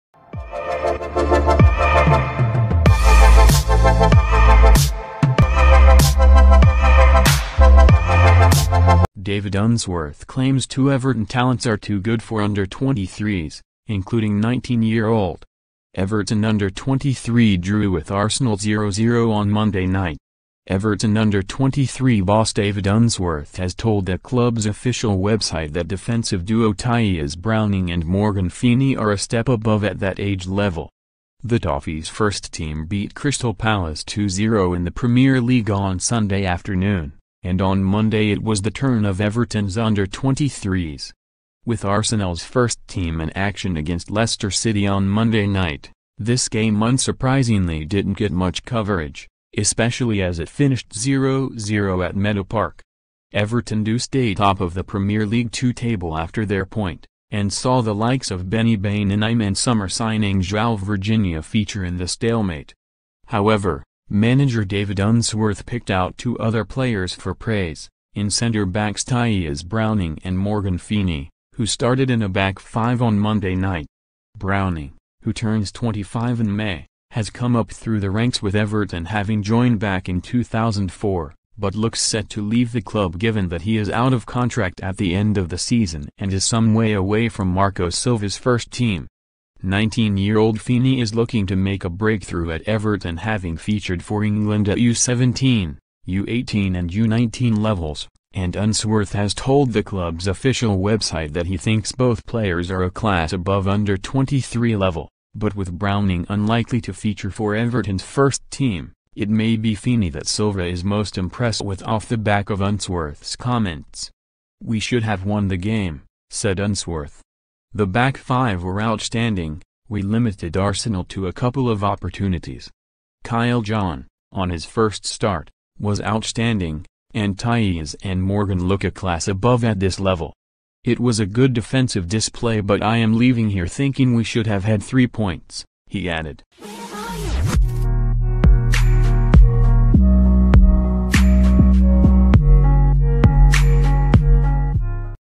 David Unsworth claims two Everton talents are too good for Under 23's; including 19 year old
David Unsworth claims two Everton talents are too good for Under-23's; including 19-year-old. Everton Under-23 drew with Arsenal 0-0 on Monday night.

Everton Under-23 boss David Unsworth has told the club's official website that defensive duo Tyias Browning and Morgan Feeney are a step above at that age level.

The Toffees first team beat Crystal Palace 2-0 in the Premier League on Sunday afternoon, and on Monday it was the turn of Everton's Under-23's.

With Arsenal's first team in action against Leicester City on Monday night, this game unsurprisingly didn't get much coverage, especially as it finished 0-0 at Meadow Park.

Everton do stay top of the Premier League 2 table after their point, and saw the likes of Beni Baningime and summer signing Joao Virginia feature in the stalemate.

However, manager David Unsworth picked out two other players for praise, in centre backs Tyias Browning and Morgan Feeney, who started in a back five on Monday night.

Browning, who turns 25 in May, has come up through the ranks with Everton having joined back in 2004, but looks set to leave the club given that he is out of contract at the end of the season and is some way away from Marco Silva's first team.

19-year-old Feeney is looking to make a breakthrough at Everton having featured for England at U17, U18 and U19 levels, and Unsworth has told the club's official website that he thinks both players are a class above Under-23 level - but with Browning unlikely to feature for Everton's first team, it may be Feeney that Silva is most impressed with off the back of Unsworth's comments.

“We should have won the game,” said Unsworth. “The back five were outstanding - we limited Arsenal to a couple of opportunities. Kyle John, on his first start, was outstanding, and Tyias and Morgan look a class above at this level. It was a good defensive display but I am leaving here thinking we should have had three points," he added.